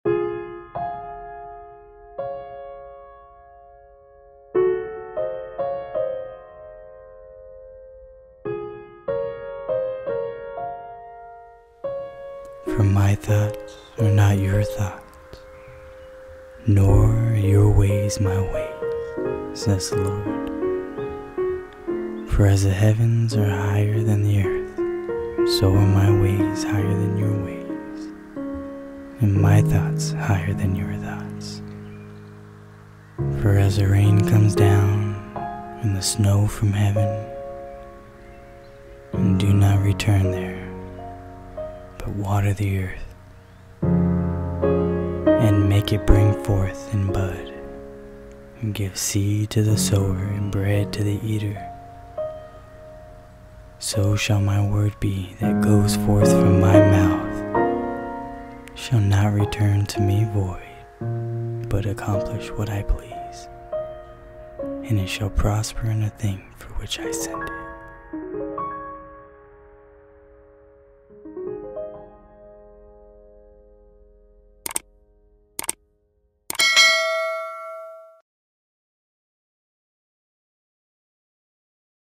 Isaiah 55:8-11 Scripture Reading | God's Word will never return to Him Empty
This is a motivational Lyric Video of Isaiah 55 to encourage you throughout your day.

Isaiah 55:8-11 (NKJV)
8“For My thoughts are not your thoughts,
Nor are your ways My ways,” says the Lord.
9 “For as the heavens are higher than the earth,
So are My ways higher than your ways,
And My thoughts than your thoughts.

10 “For as the rain comes down, and the snow from heaven,
And do not return there,
But water the earth,
And make it bring forth and bud,
That it may give seed to the sower
And bread to the eater,
11 So shall My word be that goes forth from My mouth;
It shall not return to Me [c]void,
But it shall accomplish what I please,
And it shall prosper in the thing for which I sent it.